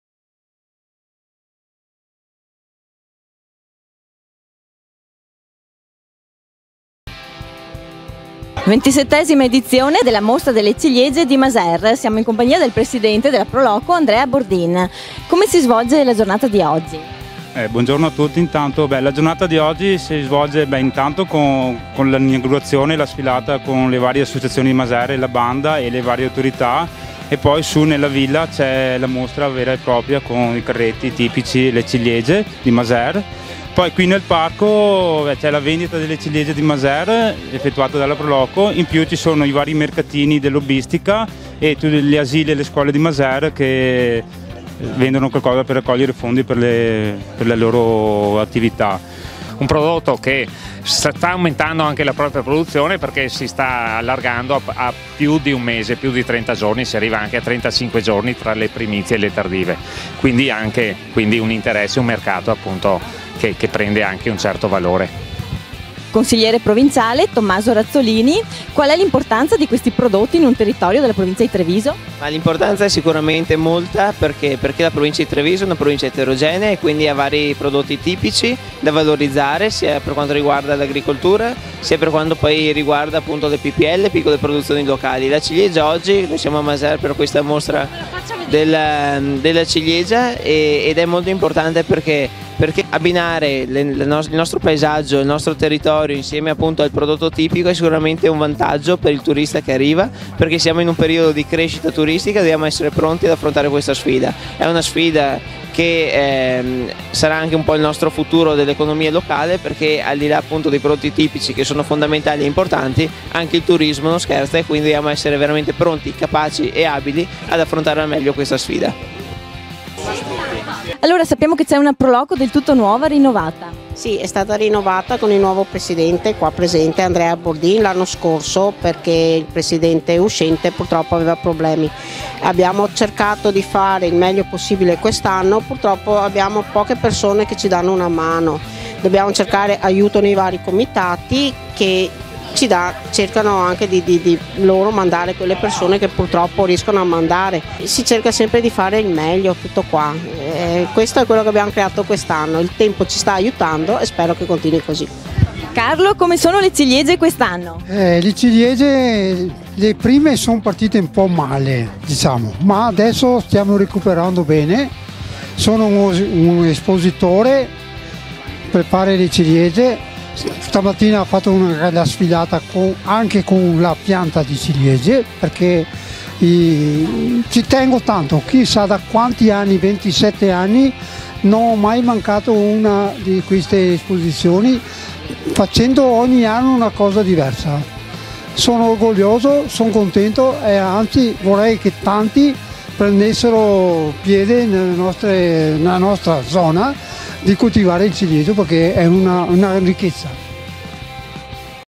Mostra della ciliega a Maser
L'ormai tradizionale appuntamento alla Mostra della Ciliegia di Maser è giunto alla 27° edizione, con il suggestivo viale dei tigli nel parco di Villa Barbaro e nelle zone limitrofe con mercatini, animazione e ciliegie!